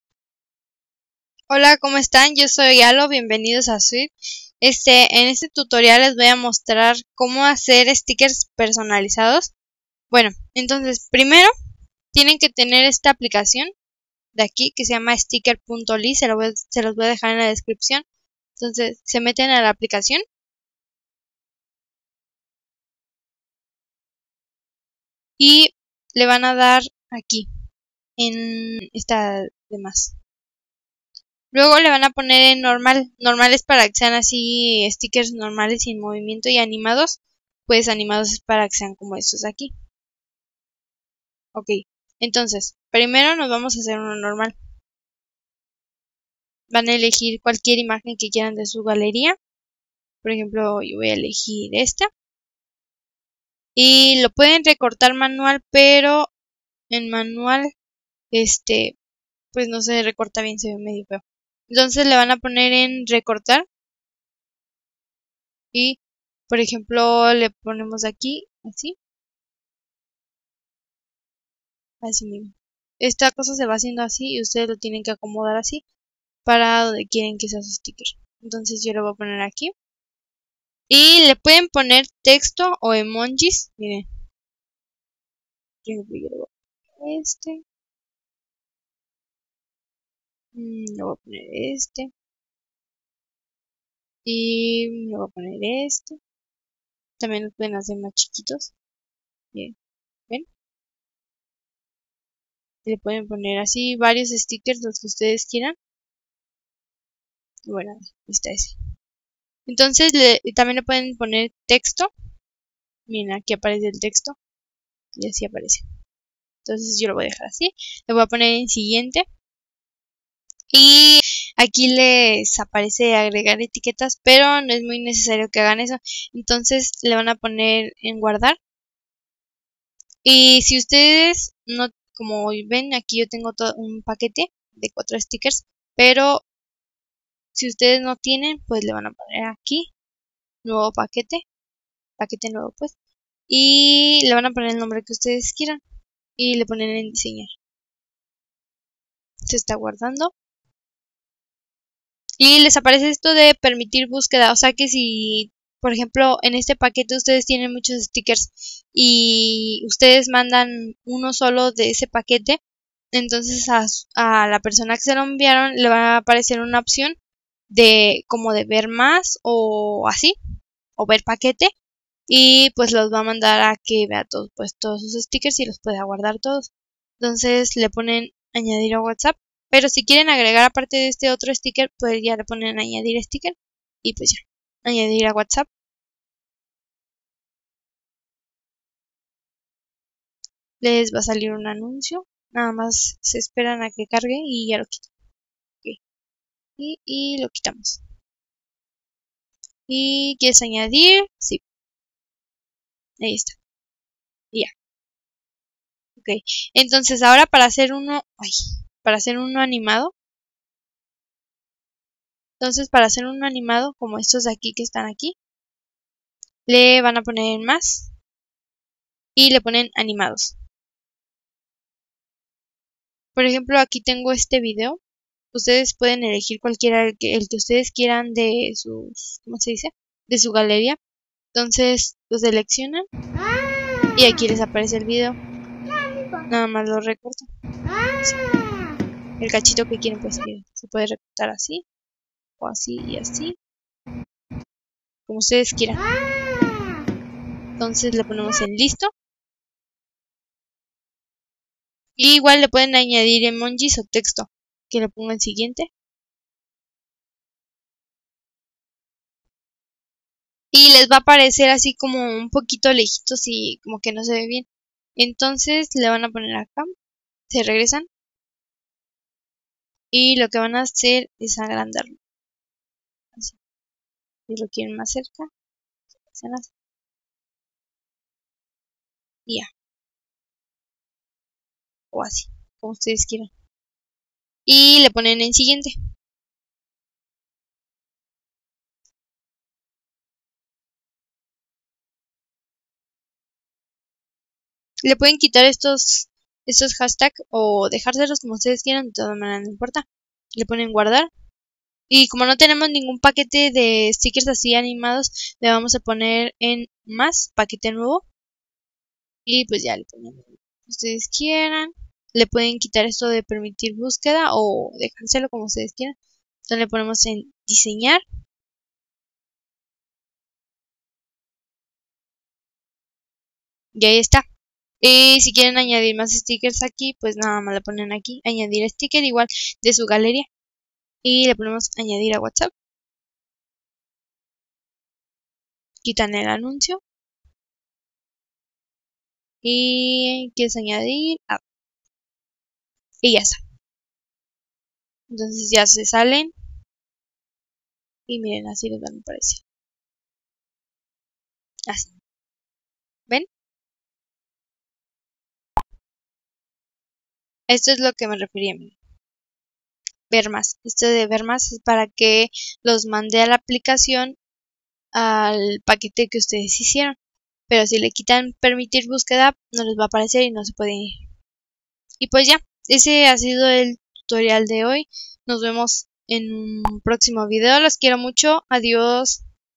CREAR STICKERS ANIMADOS para WHATSAPP!! Crea tus propios STICKERS para WHATSAPP
🐱‍👤Mi Canal de Free Fire: @zuit.x